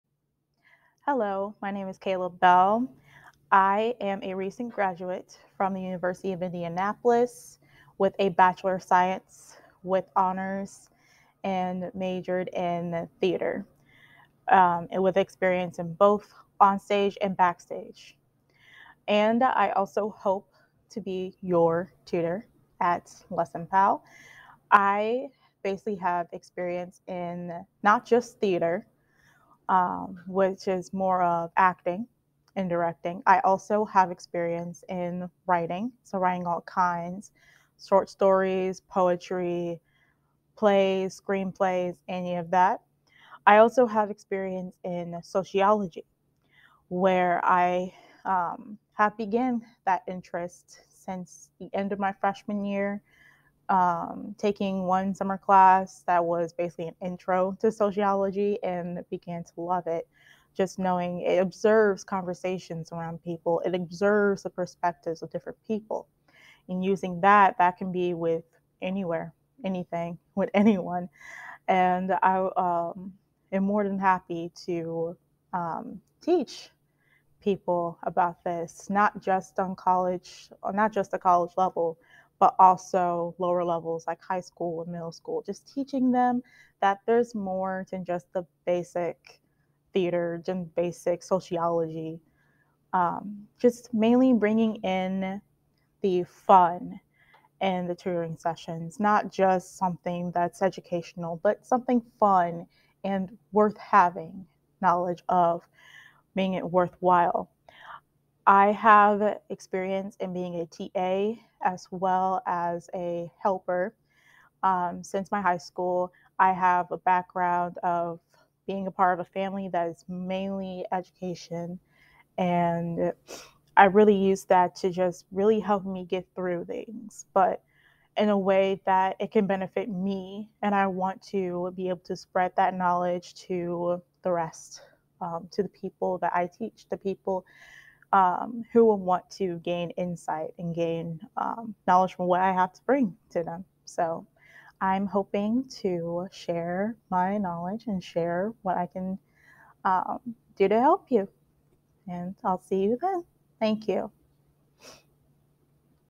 Kayla Bell - Lessonpal intro video
Kayla Bell offers lessons in Theater, Sociology, Writing and Algebra. Check out her profile and book a lesson today!